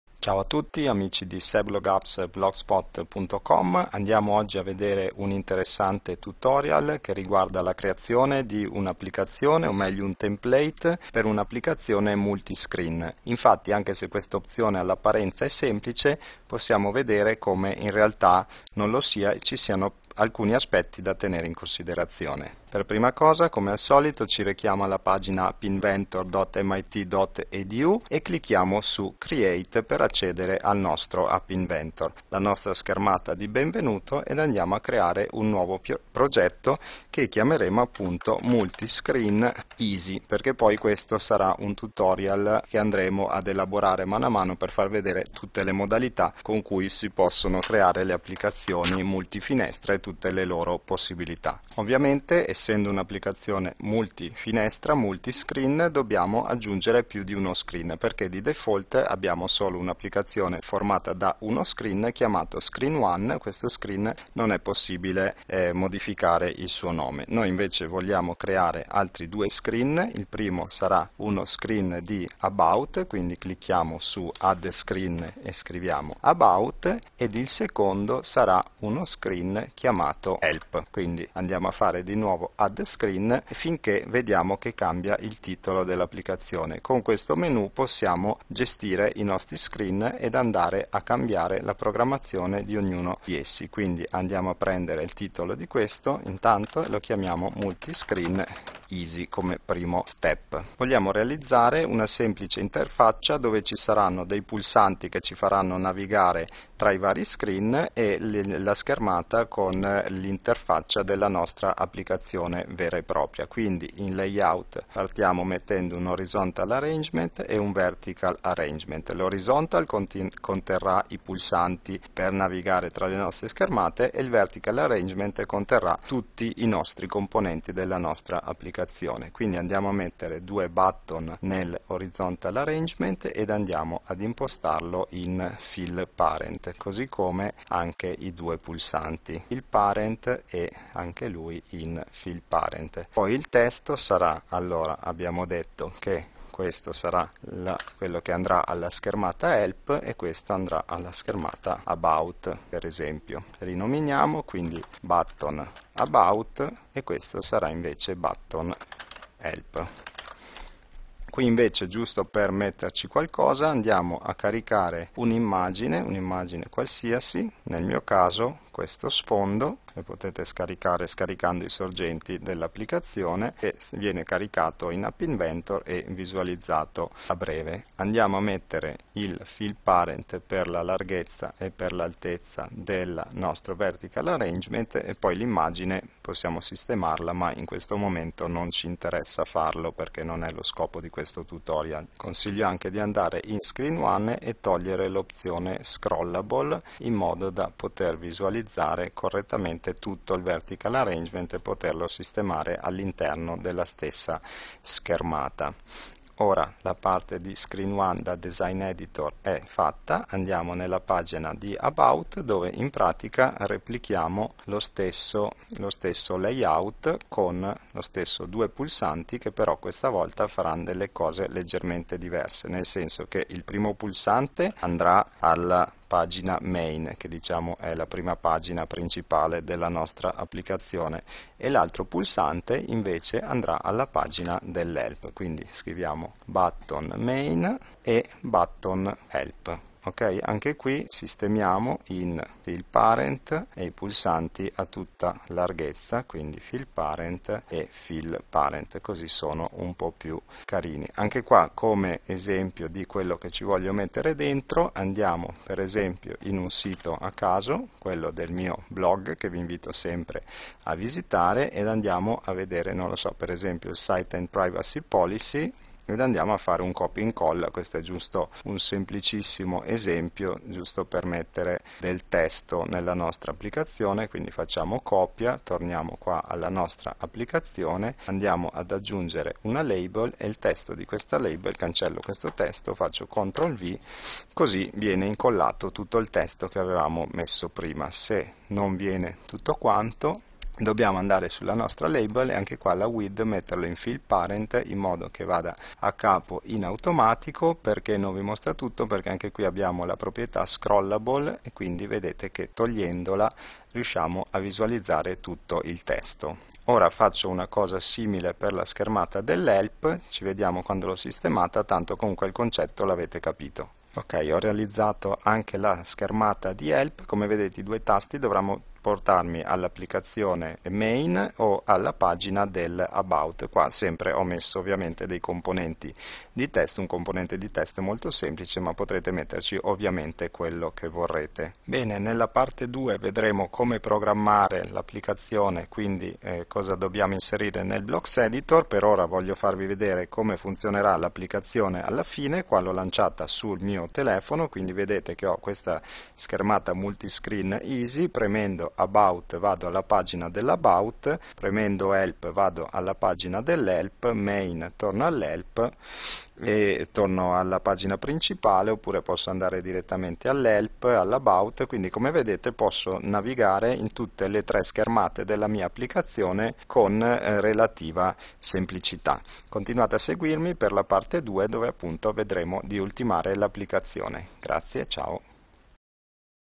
Tutorial (1/2) - Creare un applicazione MultiScreen con MIT App Inventor 2
In questo primo video dedicato al MultiScreen andremo a realizzare una semplice app composta da tre schermate diverse (Main,Help e About) e realizzeremo i pulsanti per navigare tra esse.
Nella prima parte realizziamo l'interfaccia utente dell'app.